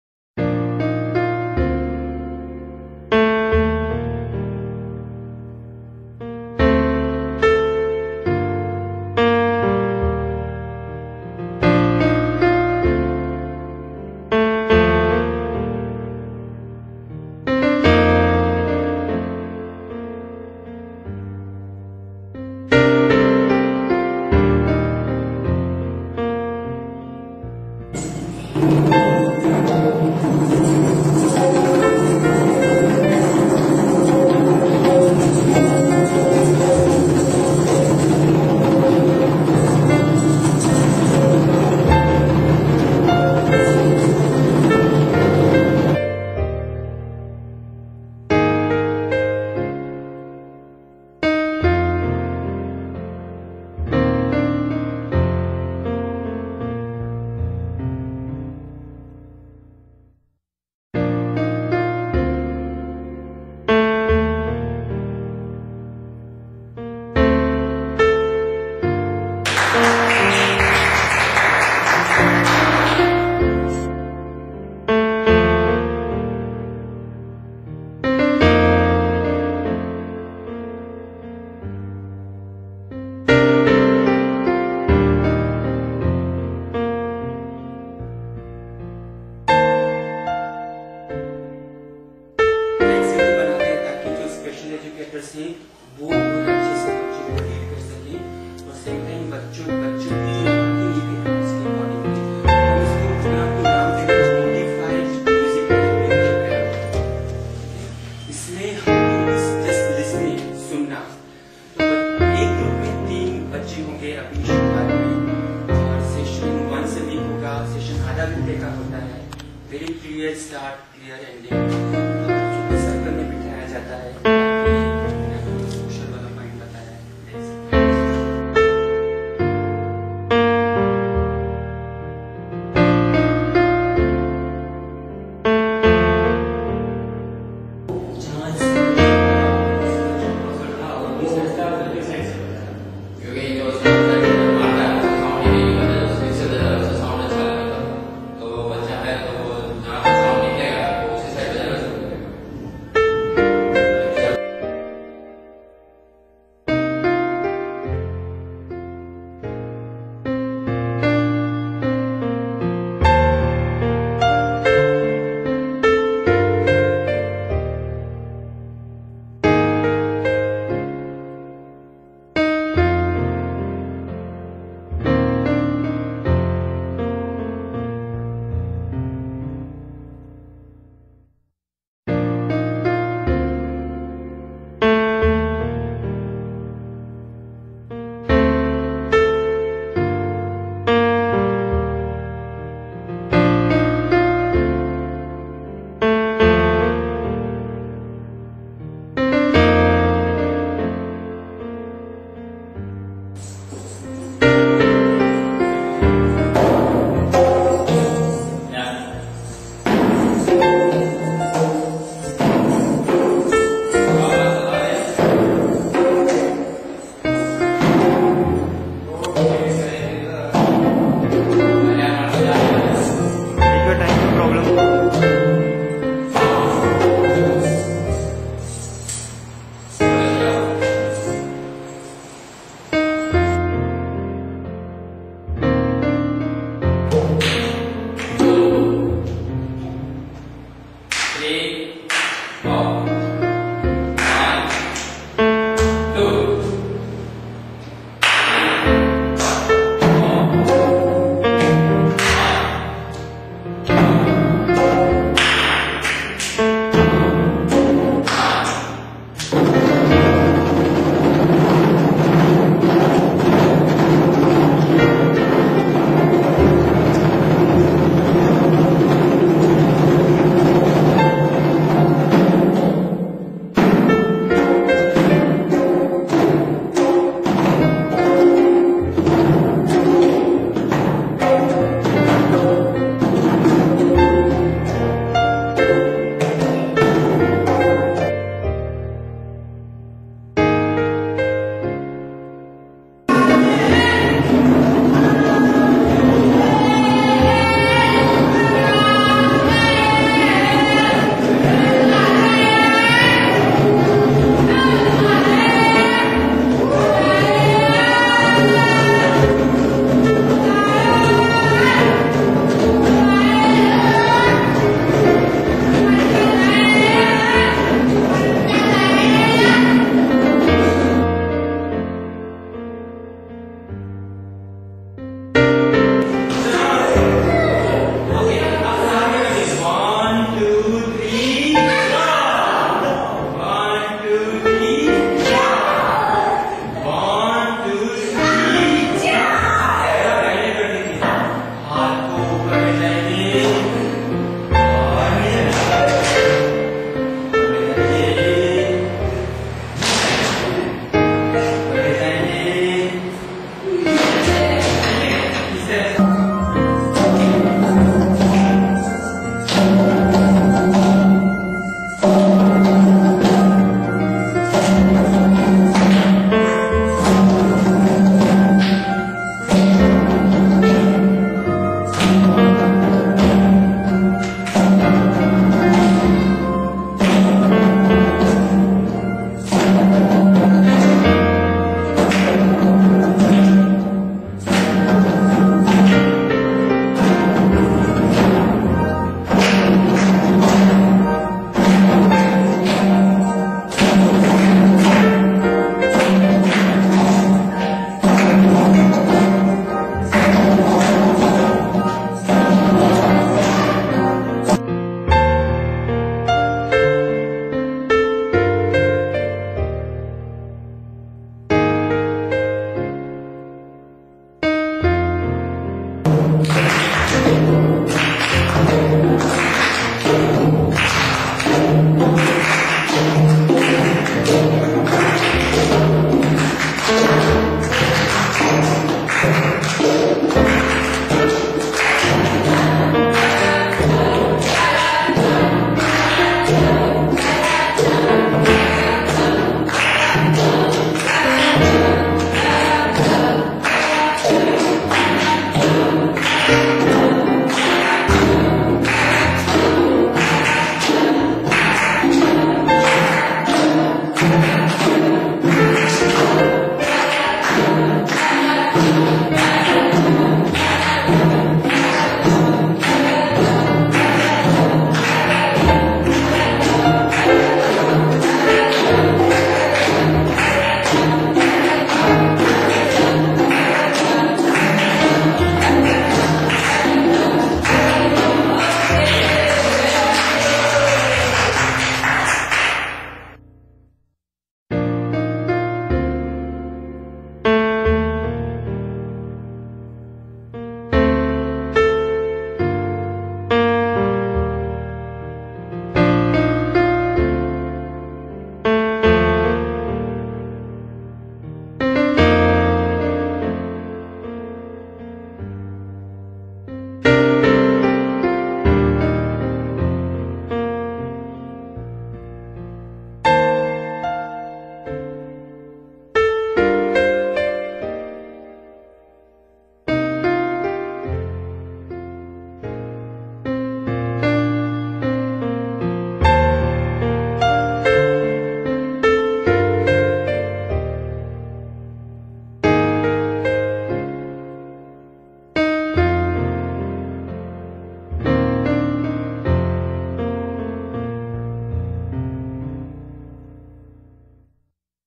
Music Therapy Program for Children with Special Needs
The Modified Music Therapy program for Special educator is a comprehensive training program to train the special educator to use music a therapeutic tool to be applied in Music club sessions for children with special needs.

Six workshops for the special educators were conducted at the Blind welfare council (BWC) Dahod, Gujarat.

The children shown in the movie lives in the residential school run by BWC and the special educators are the teaching staff member of BWC getting trained in the workshops.

Mr. Somesh Purey, a certified Music therapist have been the tutor of this project and The Head of the project is Margaret Lobo, she is the director of The Music Therapy Trust, India